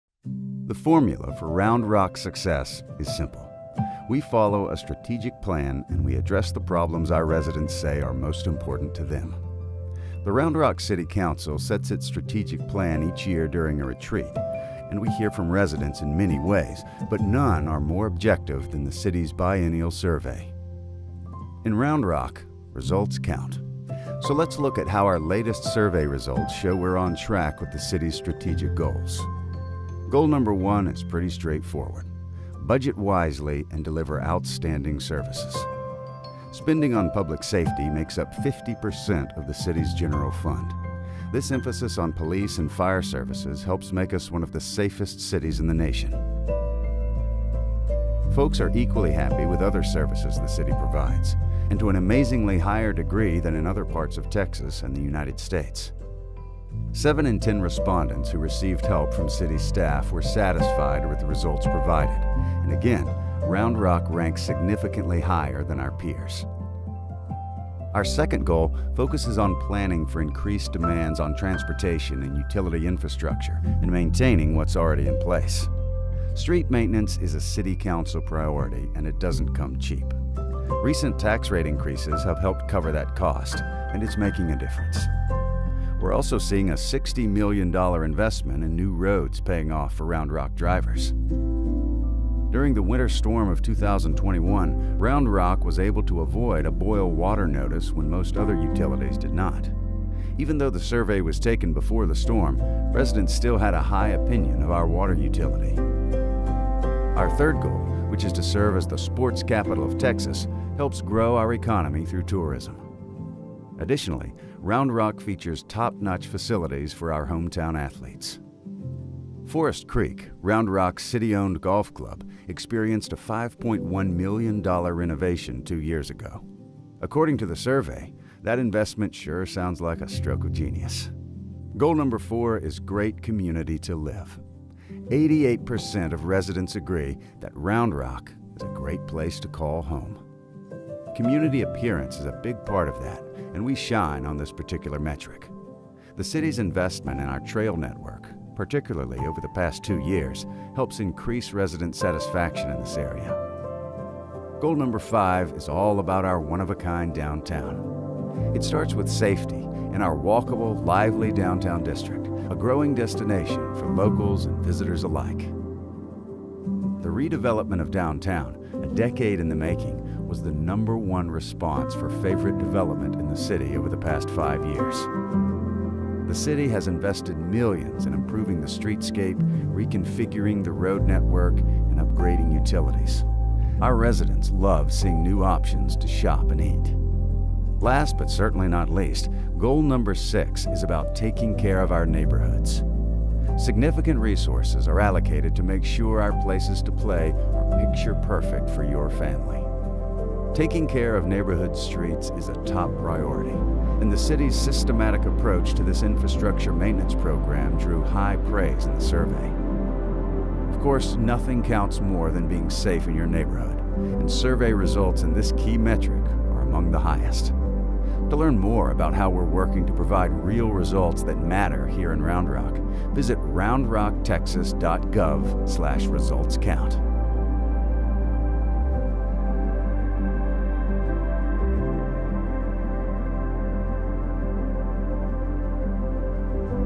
Results Count — City of Round Rock
Success in community-building requires playing the long game, especially in a fast-growing city like Round Rock. Check out the outstanding results the City received in its most recent citizen survey.